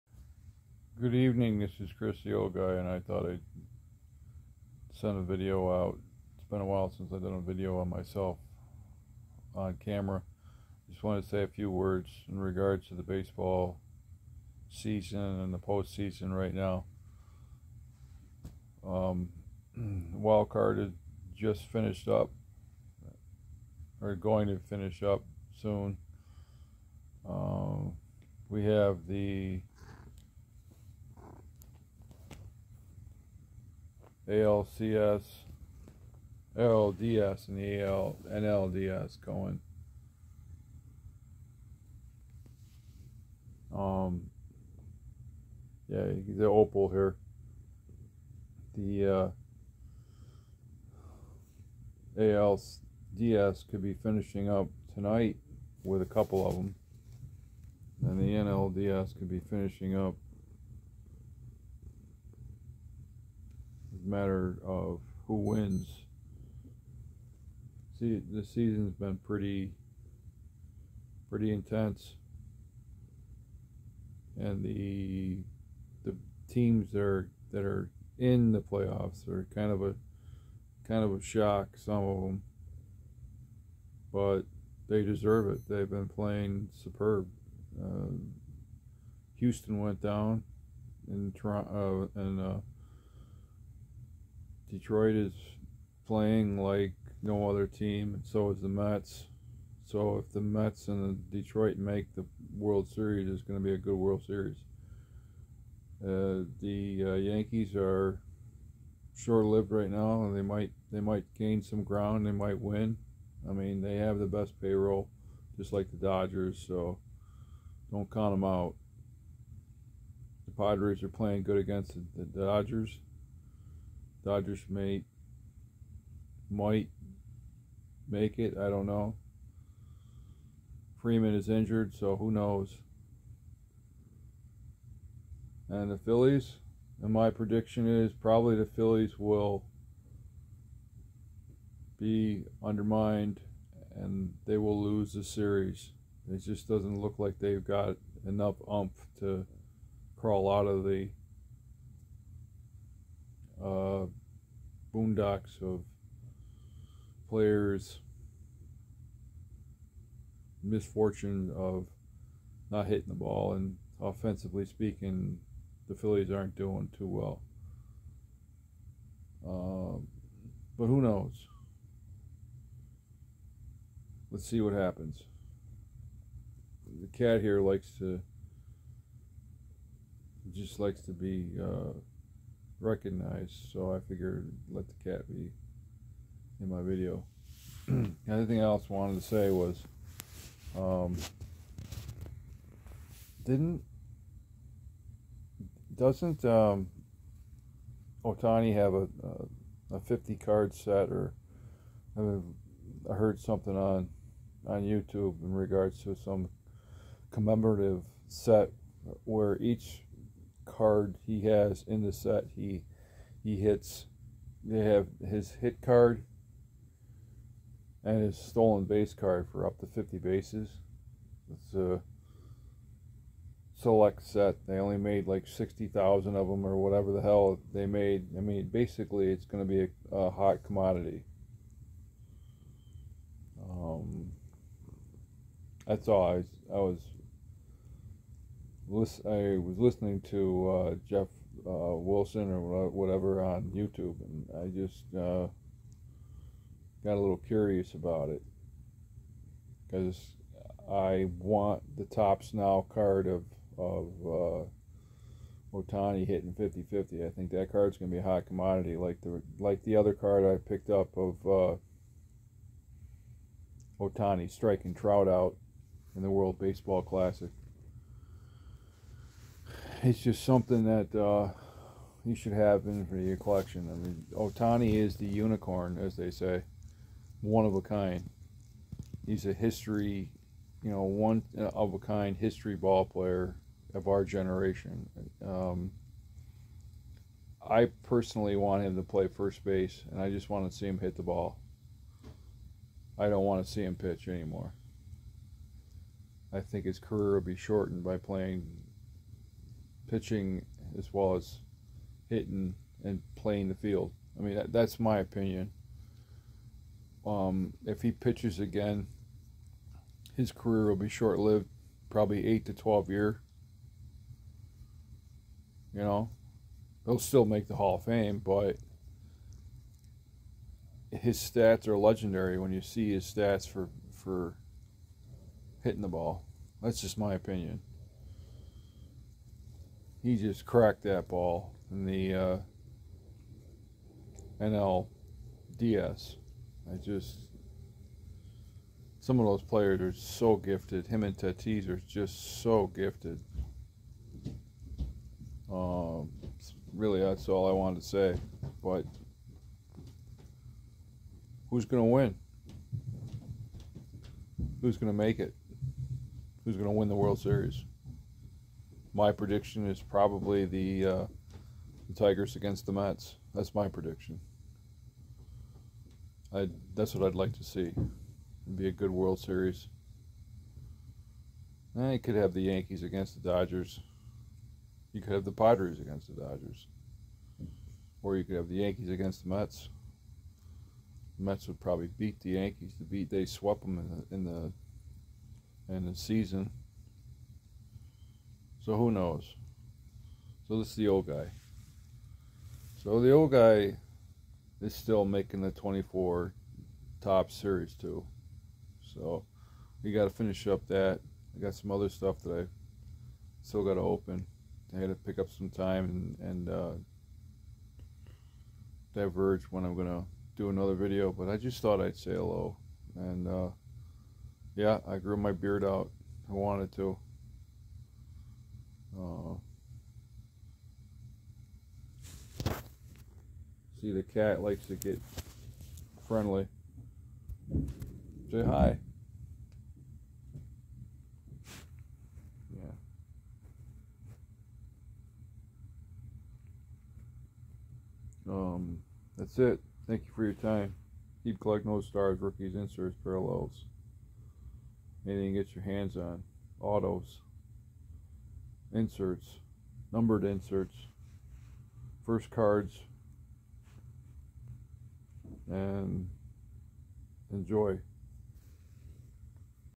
Baseball playoffs wildcard nlds also whose gonna win wth opal
I just wanted to say a few words about the basball season and it's finals along wth opal on my chair.  Whose gonna win? Will the yanks and dodgers prevail or will some other teams.  Will ohtani play for an extended period of time in his career.?  Anythoughts on his commemorative set that just came out dedicating 50 50 season? Keep collecting those autos first cards and hofers.